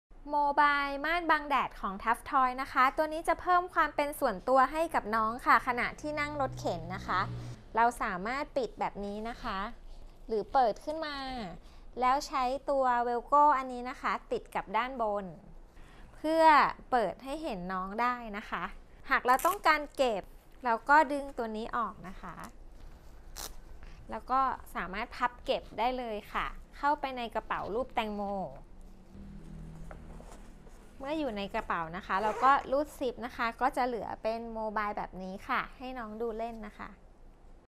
Review Taf toys โมบายม่านบังแดด Watermelon Sun Shade
ดูรายละเอียดเพิ่มเติมทาง

3 in 1 ผ้าคลุมม่านบังแดด และโมบายรถเข็นเด็ก ปกป้องลูกน้อยของคุณจาก
รังสี UVA และ UVB กว่า 97% จากรังสีที่เป็นอันตรายจากแสงแดด

พุงกลม เมืองทองธานี ดูแลเอาใจใส่โดยพนักงานมีประสบการณ์ สินค้าตัวอย่างครบ
ช็อปสะดวก เดินสบายค่ะ